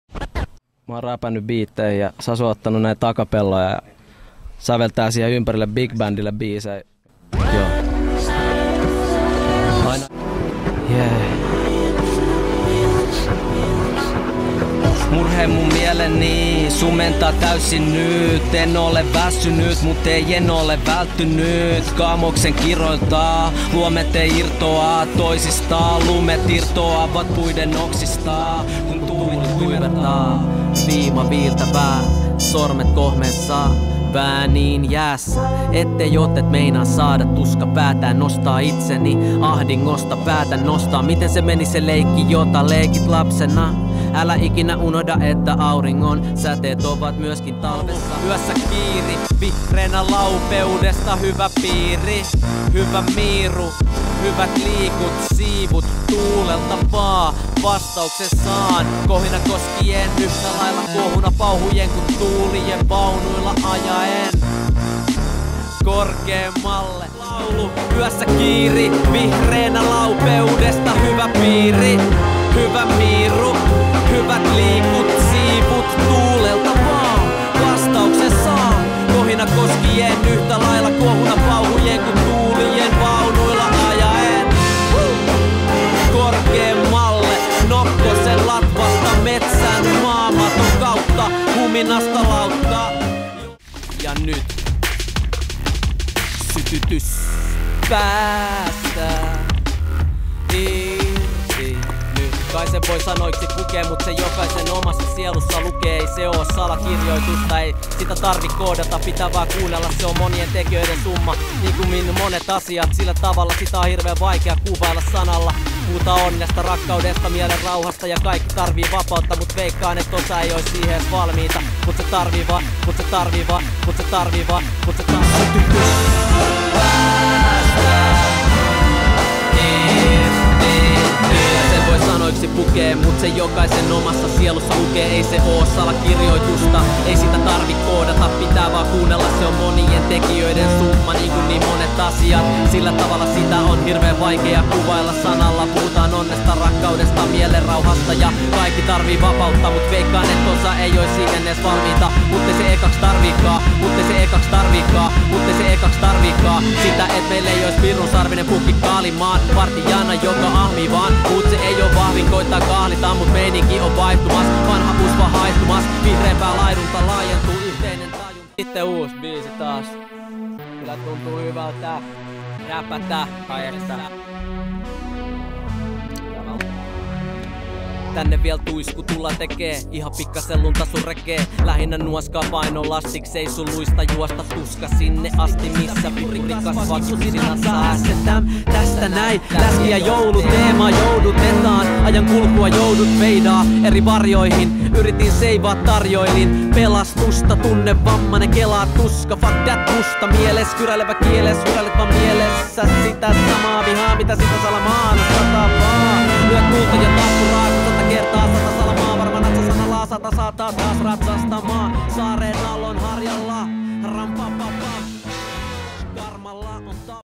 Making of 'Kuullos Orchestra with Kesken'
Kesken of rap duo Saurus recorded raps on beats and conductor Sasu Schildt composed full big band pieces around those vocals. Here are some excerpts from beat+rap versions as well as from ready compositions.

All songs composed, arranged and conducted by Sasu Schildt
All lyrics and vocals by Mikko "Kesken" Kuuluvainen
Recorded and mixed by Pirkka Räisänen

Kuullos Orchestra:

Alto Saxophones: Johannes Sarjasto, Johannes Salomaa
Tenor Saxophones: Jori Lindell, Frans Thomsson
Baritone Saxophone: Pekka Seppänen

Trumpets: Leo Kylätasku (lead), Martti Vesala, Ilkka Arola, Tero Savolainen

Trombones: Janne Vihavainen, Markus Larjomaa, Johannes Klementtinen, Mikko Haanpää

Rhythm Section:
Guitar - Mikko Korhonen
Piano/Rhodes - Aapo Heinonen
Bass - Jukka Peinola
Drums - Joonas Kaikko